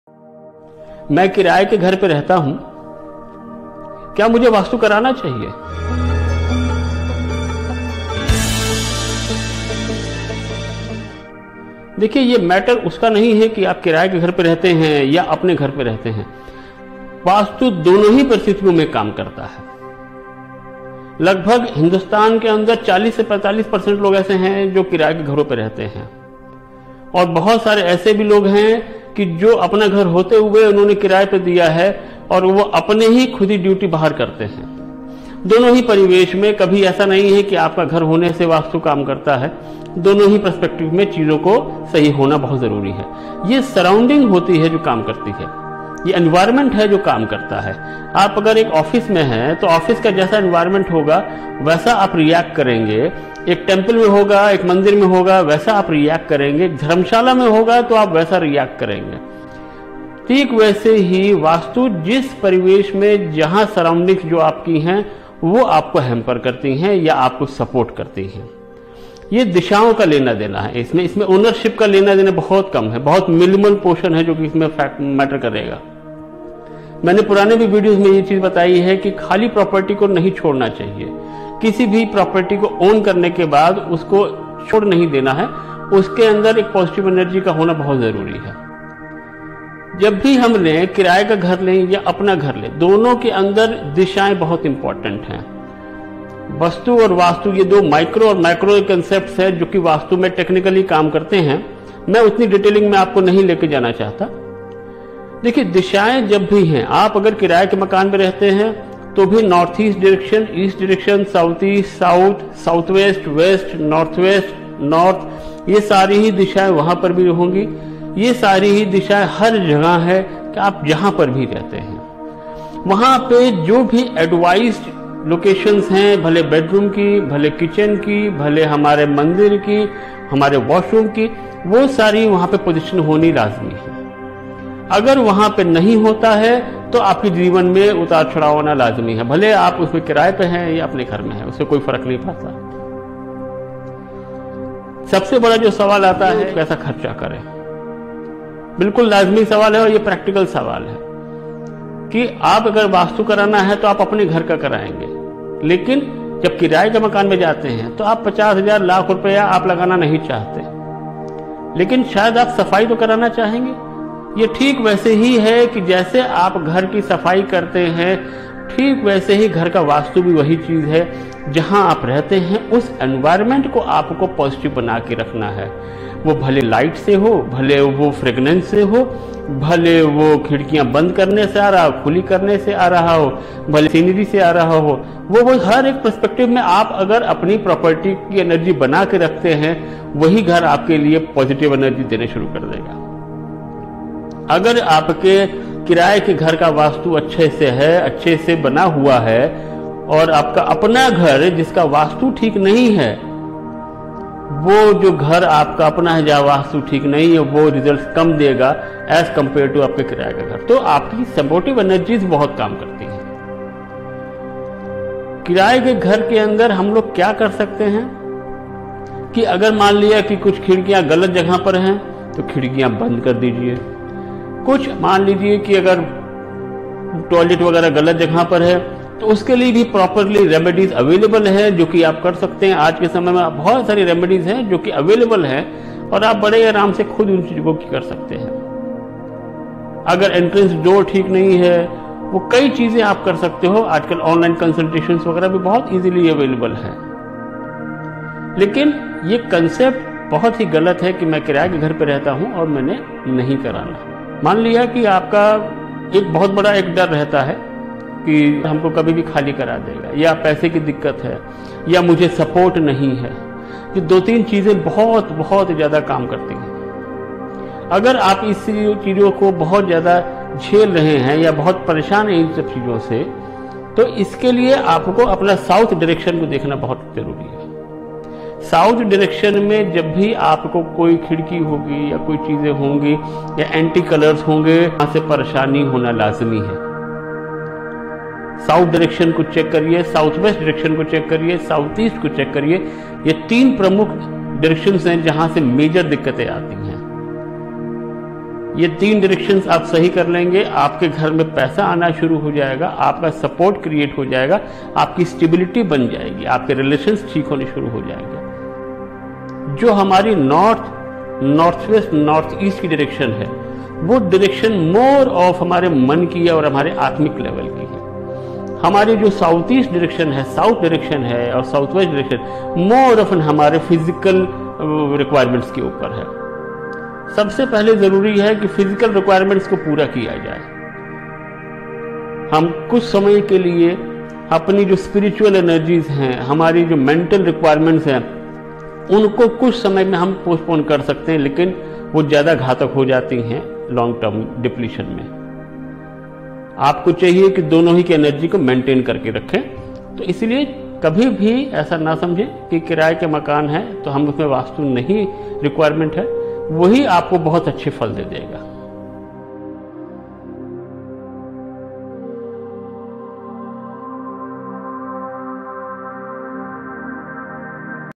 Rented spaces & Vastu Advice I Hindi Video I Aroma Vastu
Vastu in a rented property is an important debate.

Tenant when troubled and after circumstantial emergencies, he is bound to execute the Vastu process.
Aroma Vastu has best solution for Vastu corrections for rented places

This aroma Vastu film demystifies the use of essential oils in the most scientific manner as never before.
Aroma Vastu aligns various energies using natural essential oils, and not traditional Vastu solutions. The result is effective Vastu for homes, offices, and factories.

Varanasi office assistance: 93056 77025, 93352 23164